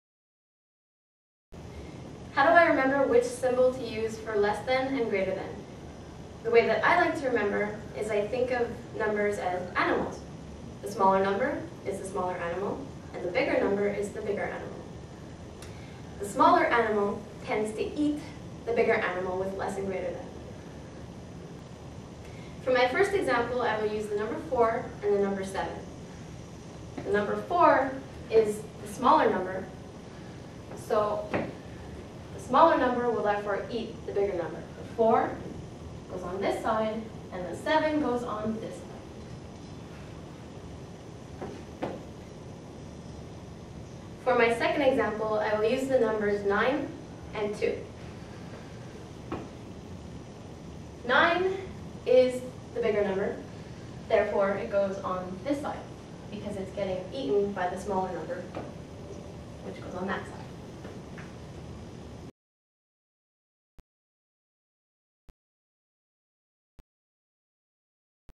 Less Than Eats Greater Than
This video shows you a trick for placing the correct number on the correct side of the Less Than and Greater Than symbols. For part 2 of this video, including examples 3 and 4, as well as many more instructional Math videos and exercise and answer sheets, go to: